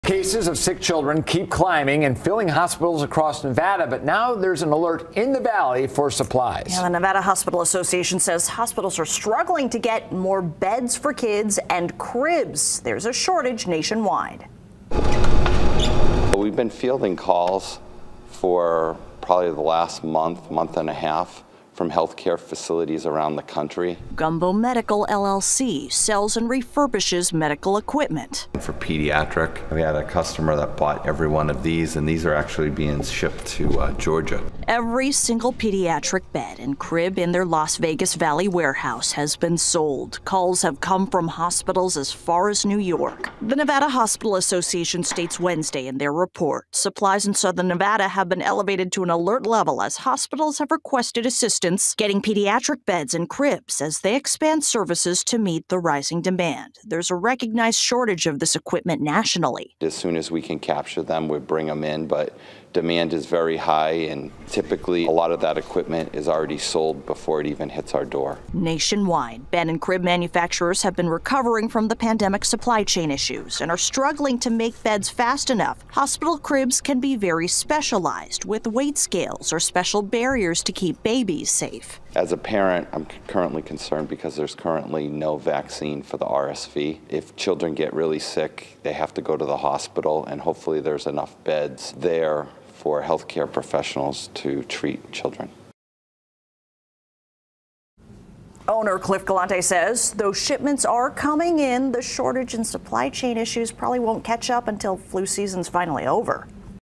Shortage of hospital cribs, pediatric beds in Southern Nevada as RSV cases climb
The rise of RSV cases across Southern Nevada has led to a shortage of hospital cribs and pediatric beds, according to the Nevada Hospital Association.